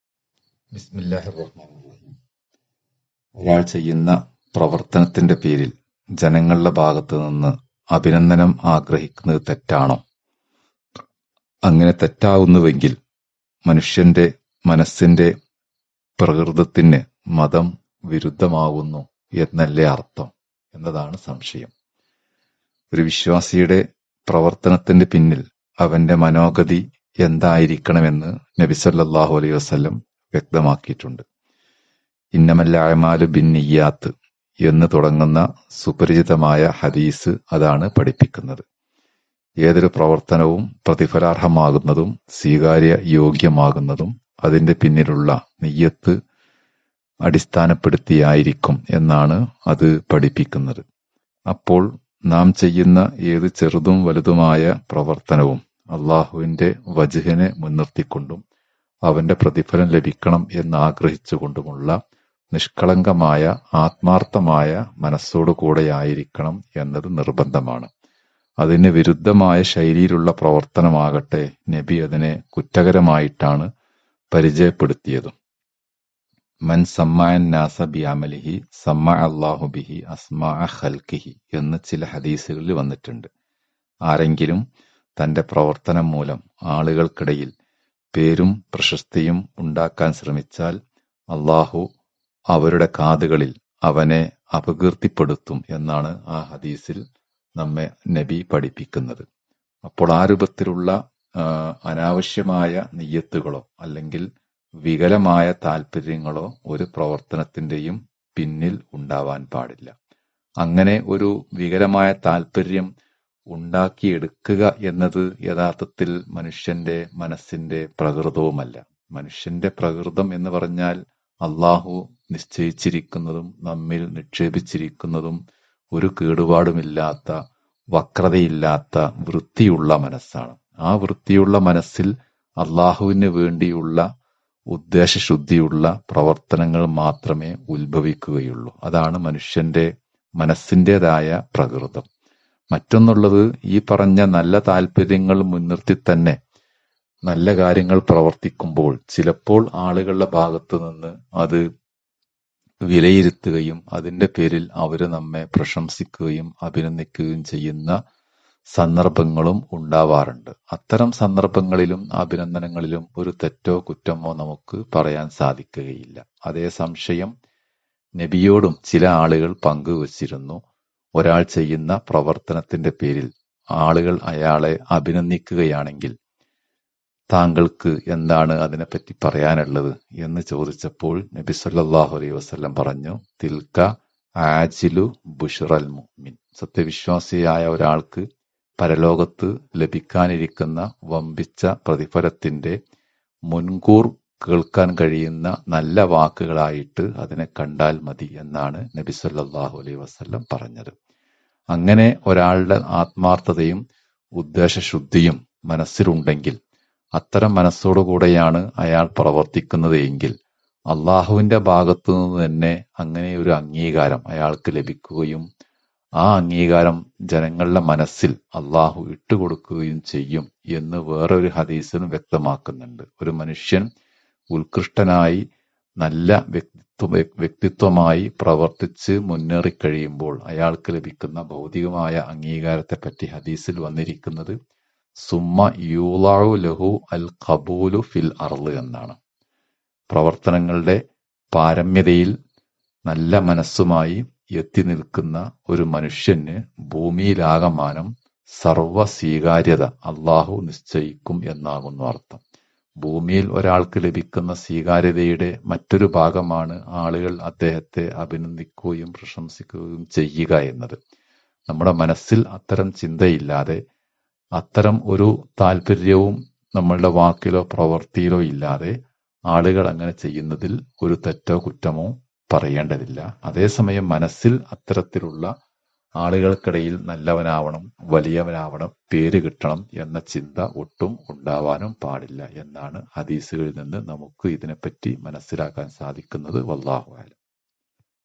സൽകർമങ്ങളുടെ പേരിൽ അഭിനന്ദനം ആഗ്രഹിക്കാമോ? | Dr. Jamaluddeen Farooqi
സൽകർമങ്ങളുടെ പേരിൽ മറ്റുള്ളവരിൽ നിന്ന് അഭിനന്ദനം ആഗ്രഹിക്കുന്നത്  തെറ്റായ കാര്യമാണോ? തെറ്റാണെങ്കിൽ, ഇസ്‌ലാം മനുഷ്യപ്രകൃതിയുടെ താല്പര്യങ്ങൾക്ക് എതിരാണെന്ന് വരില്ലേ?